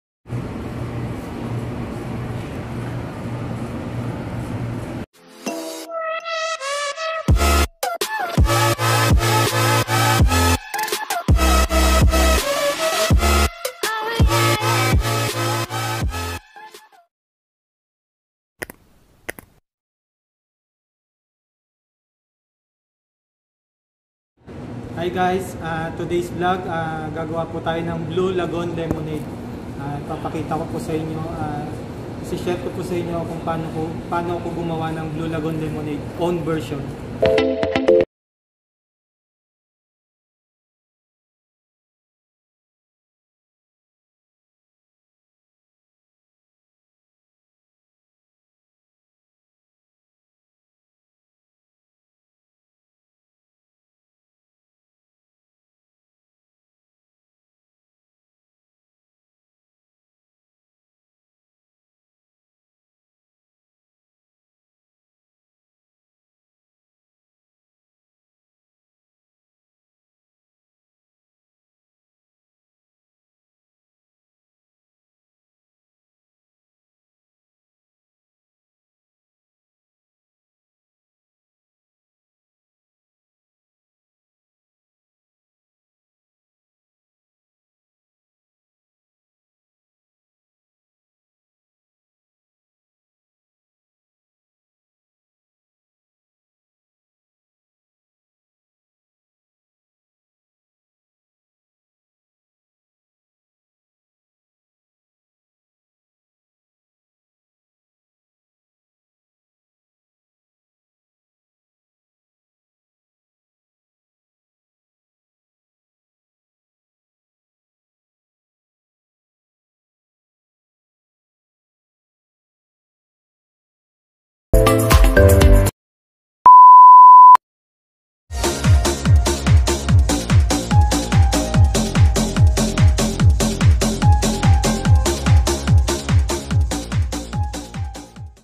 HOW TO MAKE BLUE LAGOON LEMONADE | Mixologist JanJan | Juices & Cocktails EP01
Hi Guys! I'm JanJan Tiangha
I just want to share my own version of Blue Lagoon Lemonade. Thank You!